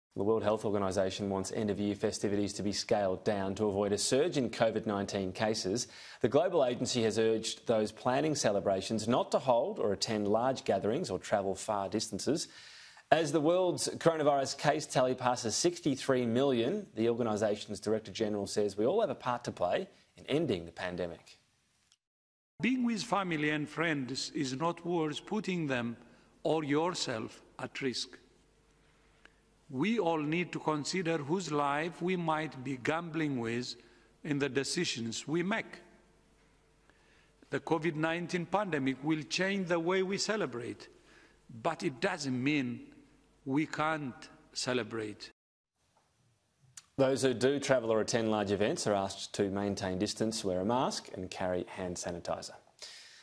WHO issues Christmas COVID-19 warning
The World Health Organisation has warned a surge in coronavirus cases could be triggered by end-of-year festivities.

The UN agency's Director-General Tedros Adhanom Ghebreyesus urged event organisers to avoid holding large gatherings and extensive travel.

“Being with family and friends is not worth putting them or yourself at risk, we all need to consider whose life we might be gambling with in the decisions we make," he said.

The global coronavirus case tally has passed 63 million.